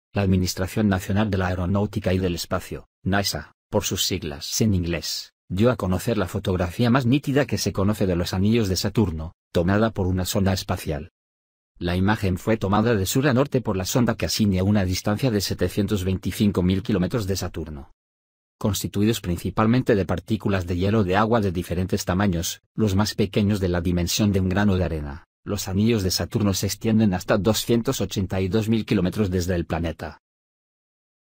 NASA revela la foto más nítida de los anillos de Saturno
La Administración Nacional de la Aeronáutica y del Espacio (NASA, por sus siglas en inglés) dio a conocer la fotografía más nítida que se conoce de los anillos de Saturno, tomada por una sonda espacial.

La imagen fue tomada de sur a norte por la sonda Cassini a una distancia de 725 mil kilómetros de Saturno.
Constituidos principalmente de partículas de hielo de agua de diferentes tamaños, los más pequeños de la dimensión de un grano de arena, los anillos de Saturno se extienden hasta 282 mil kilómetros desde el planeta.